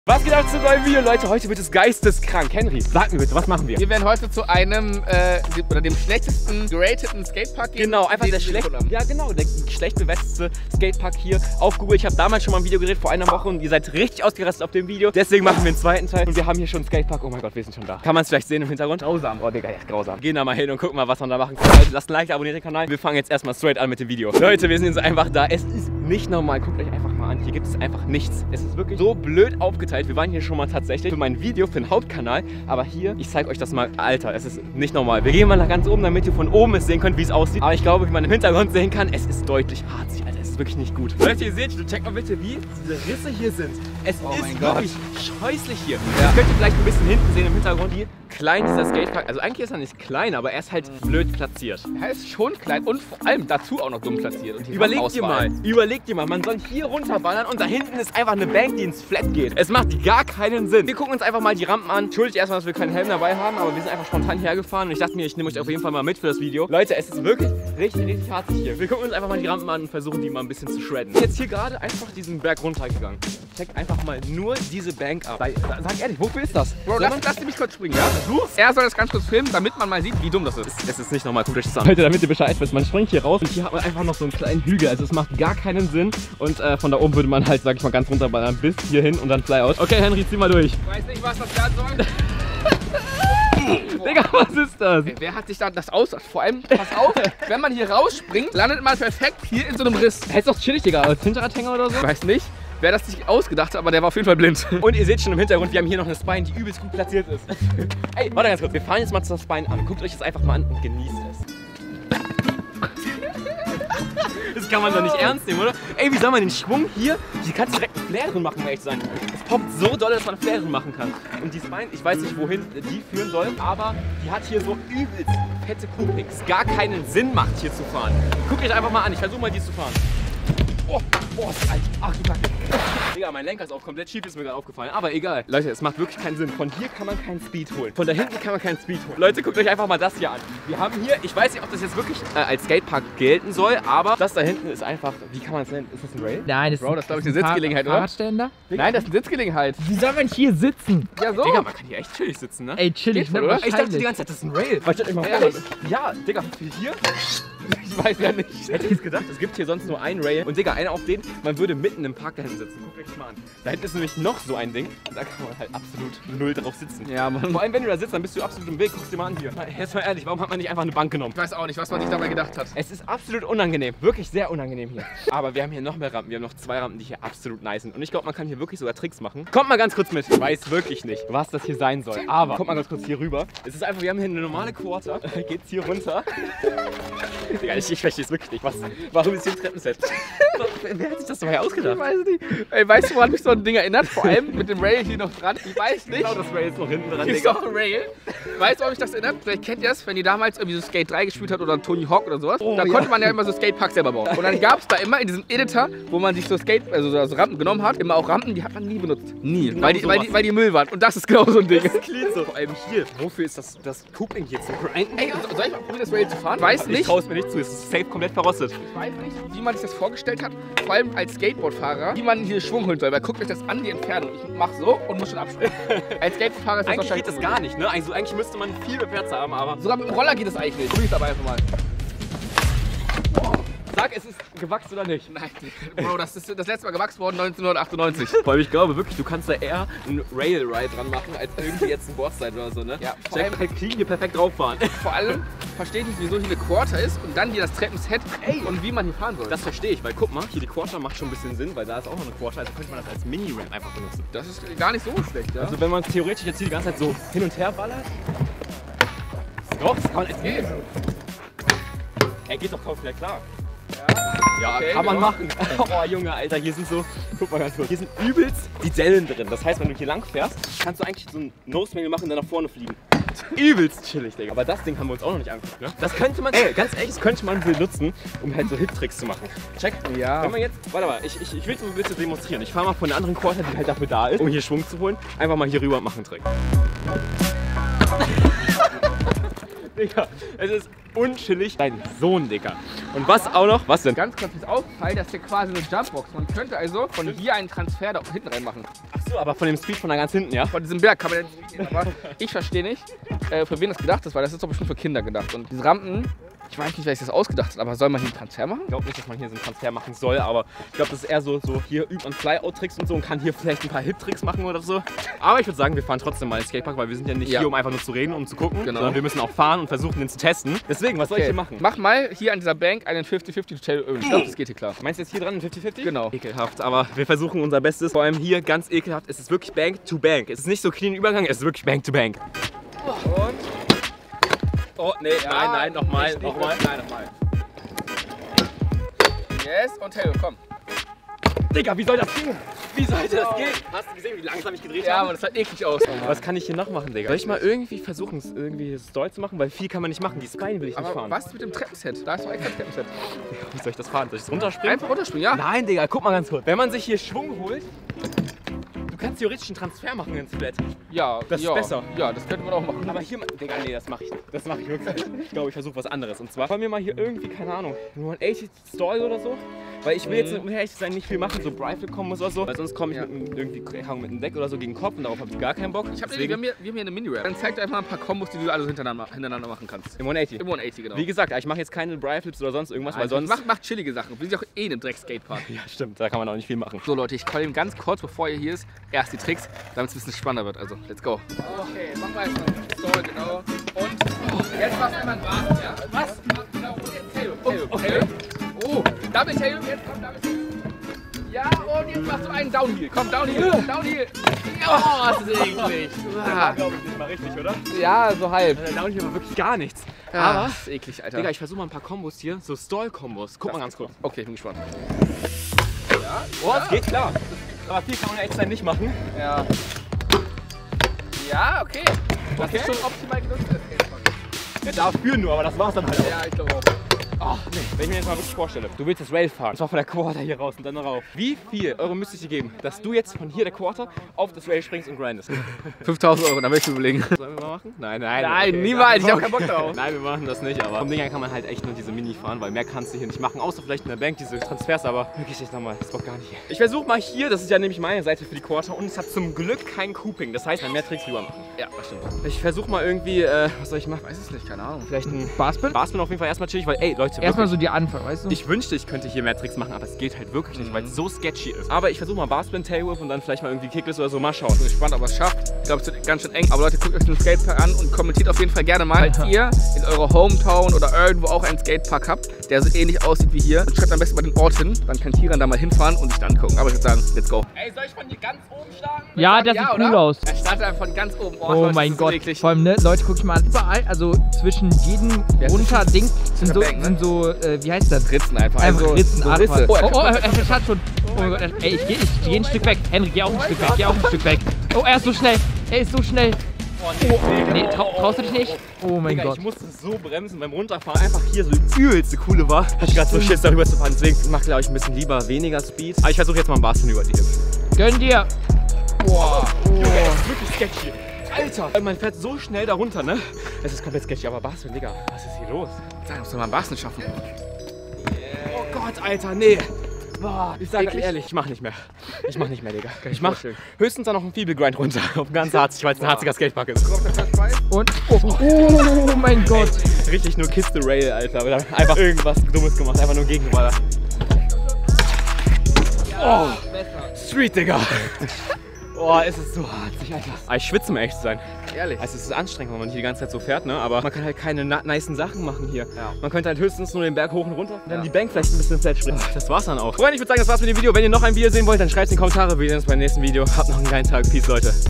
Google's Schlecht Bewertester Skatepark!
Der zweite Teil von Der SCHLECHT BEWERTESTE SKATEPARK auf Google!
Was haltet ihr von dem Skatepark?😂

Auch bei TikTok bin ich am Start!